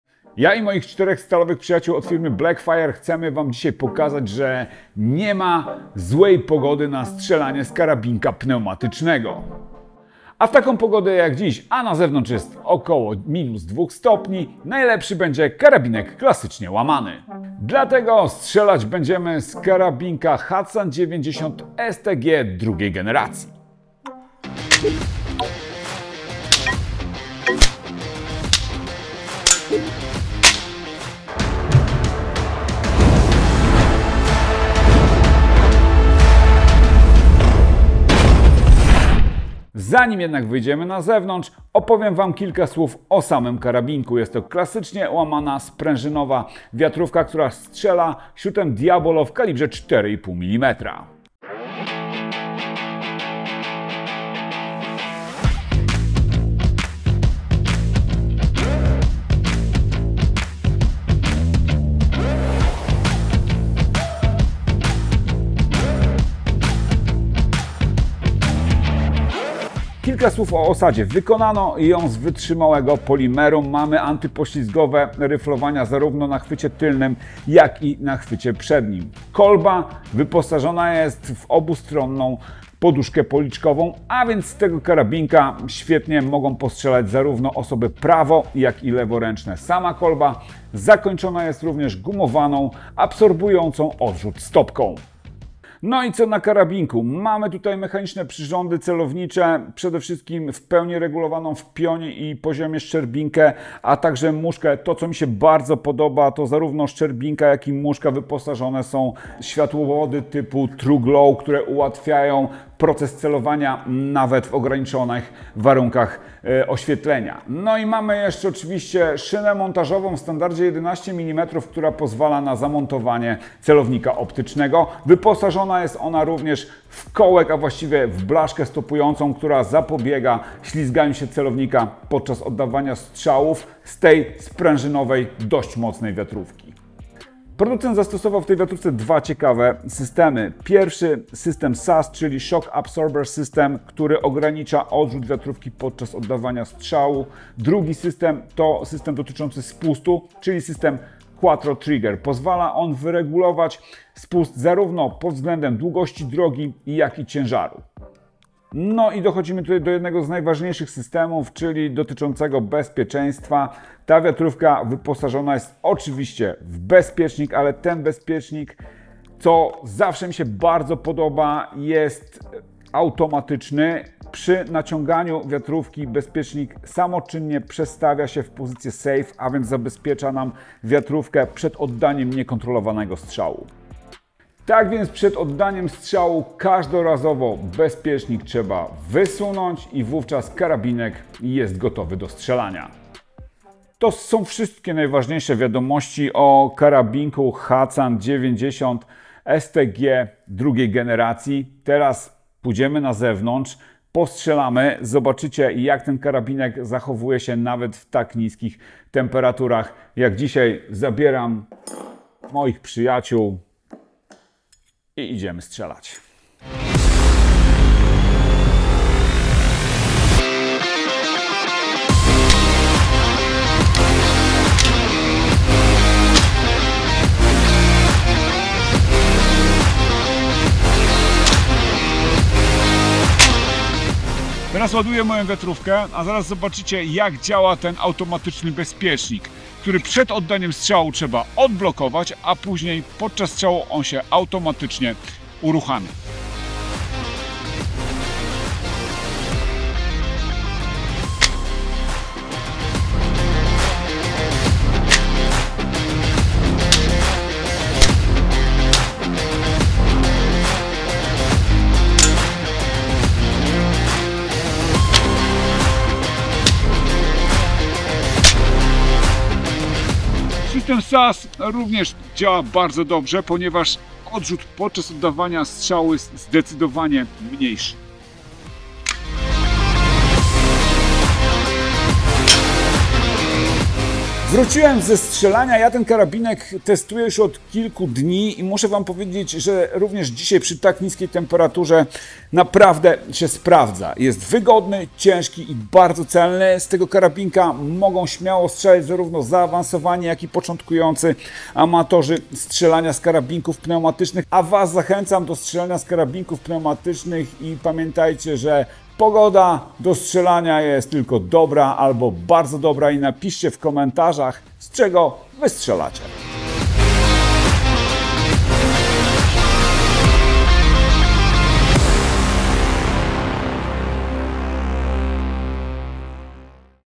Wiatrówka dla każdego i na każde warunki - Hatsan 90 STG Gen II | Sklep Militaria.pl
Do kolejnych testów strzelania wybraliśmy wiatrówkę Hatsan 90 STG drugiej generacji, bo wiedzieliśmy, że dobrze poradzi sobie przy ujemnej temperaturze. Zapraszamy na materiał, w którym Bartek szeroko omawia i sprawdza możliwości tej klasycznie łamanej wiatrówki sprężynowej.

Dane techniczne
Model: Hatsan 90 STG Gen II
Kaliber: 4,5 mm
Energia kinetyczna: poniżej 17 J
Prędkość początkowa: 305 m/s
Długość całkowita: 1135 mm
Długość lufy: 450 mm
Waga: 3400 g

00:00 Wstęp
00:40 Wykonanie
01:37 Przyrządy celownicze
02:17 System SAS i Quattro Trigger
03:37 Strzelamy przy -2 stopniach Celsjusza!
04:45 Podsumowanie
05:05 Zakończenie